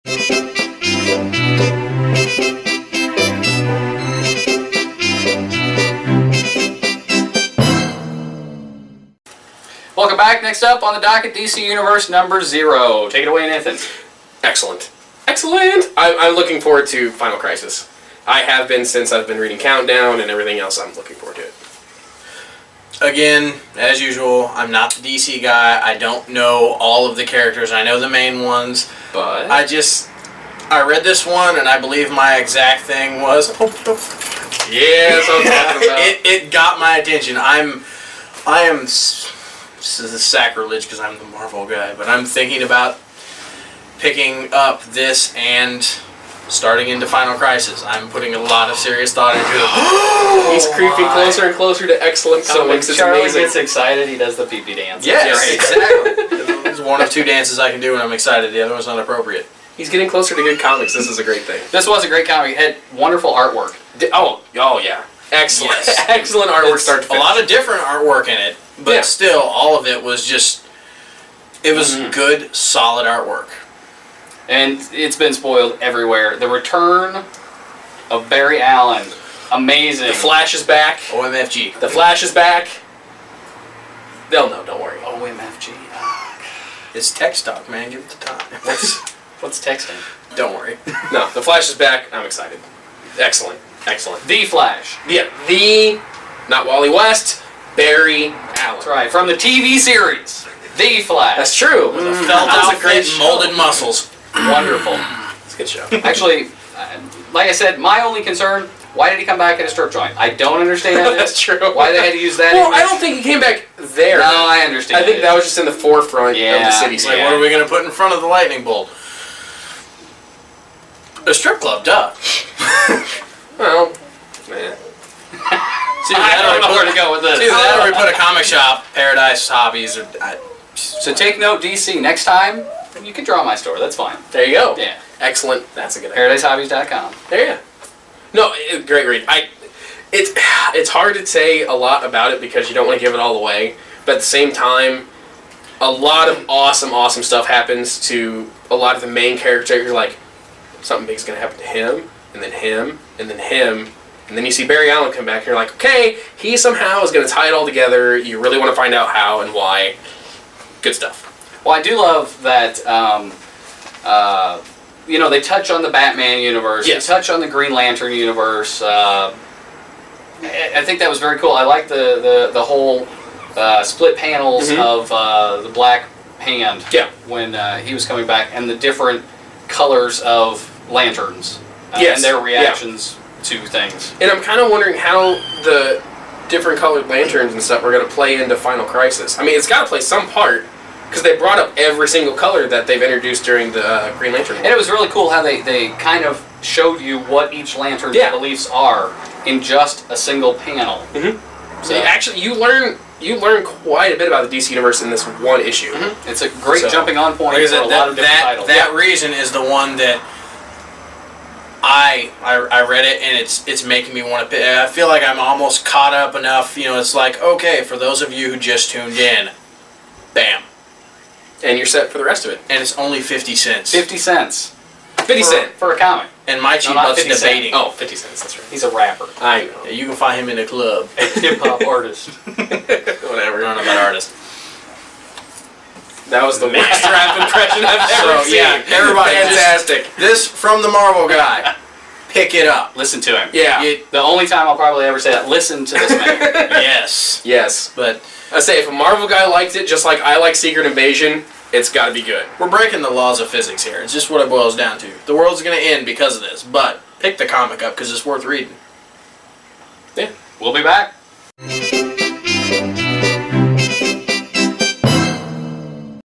Geeks Like Us, Ep. 4, Part 4
This time, our gang reviews "DC Universe 0".

for all of your comic & game needs.